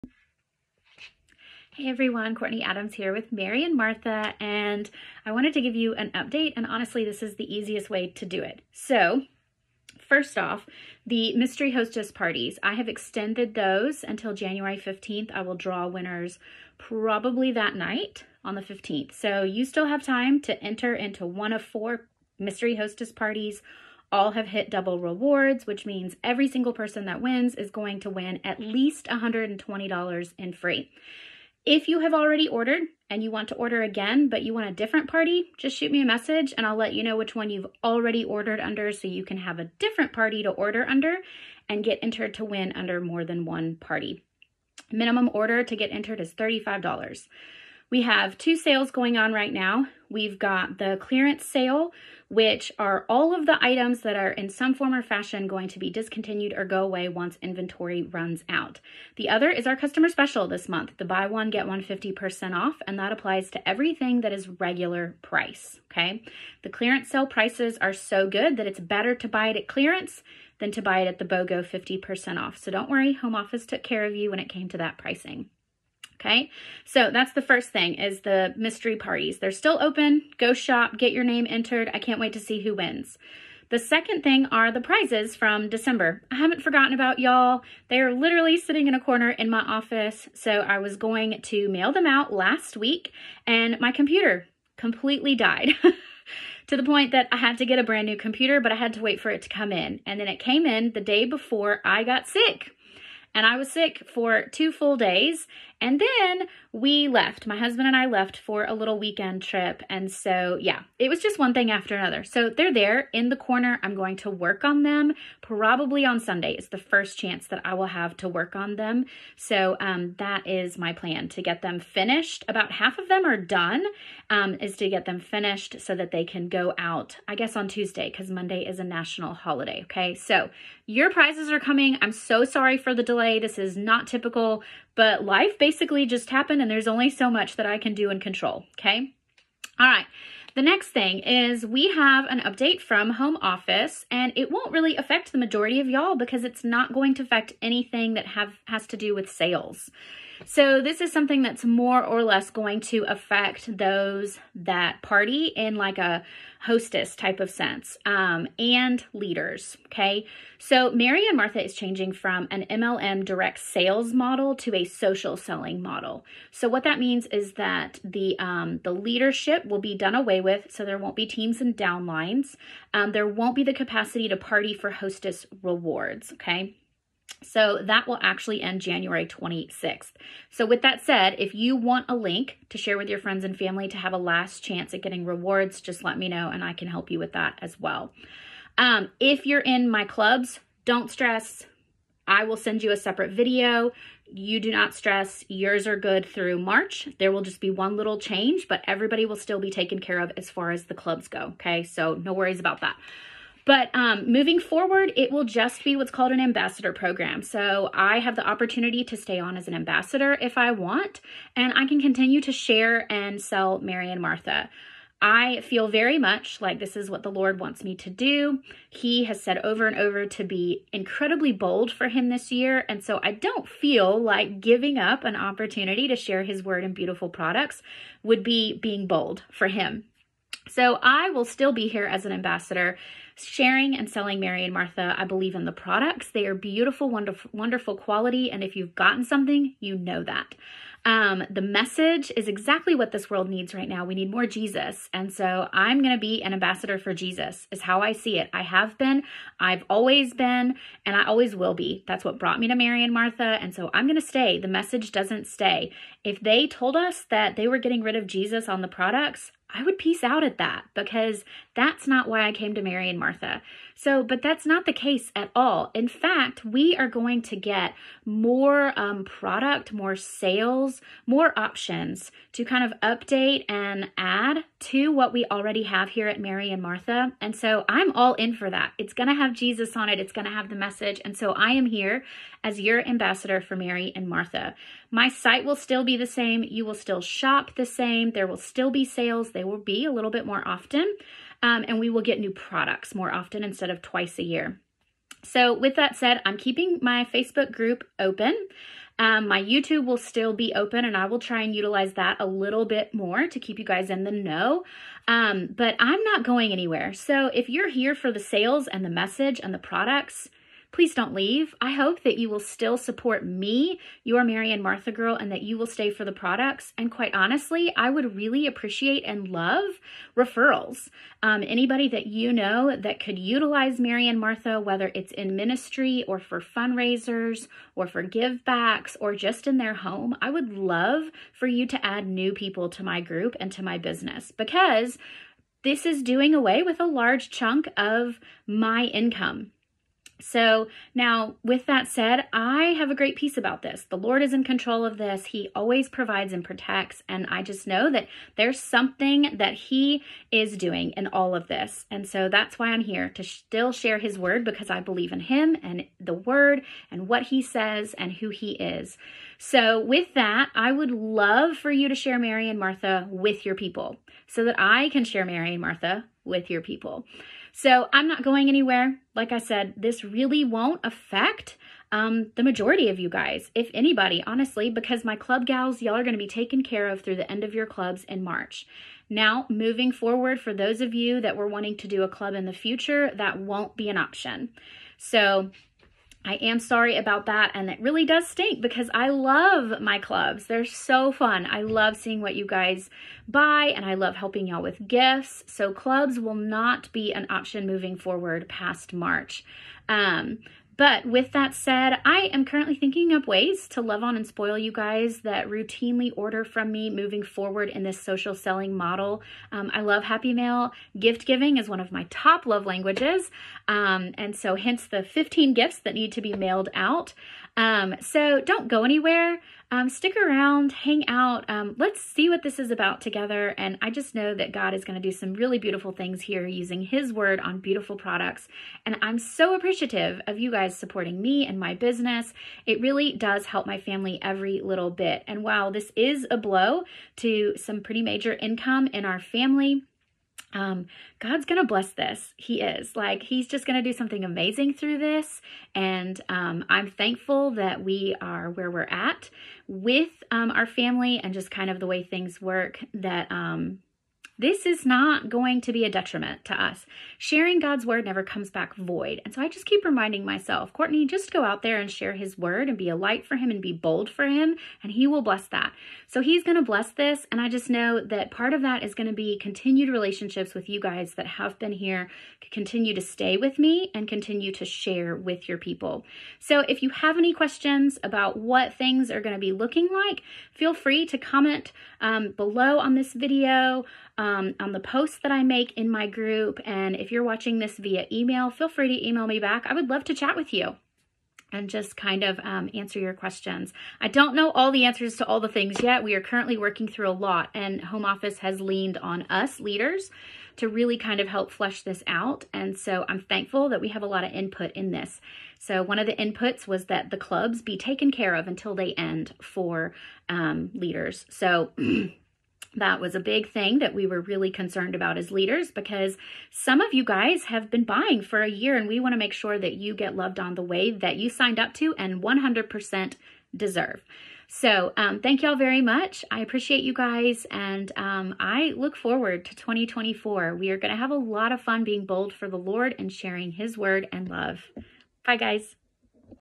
Mary & Martha update
2024 is a new year and with that come some changes. I hope you will take a minute to watch this video about what to come with Mary & Martha. And let’s go into 2024 being bold sharing the love in the word of our Lord and Savior. ￼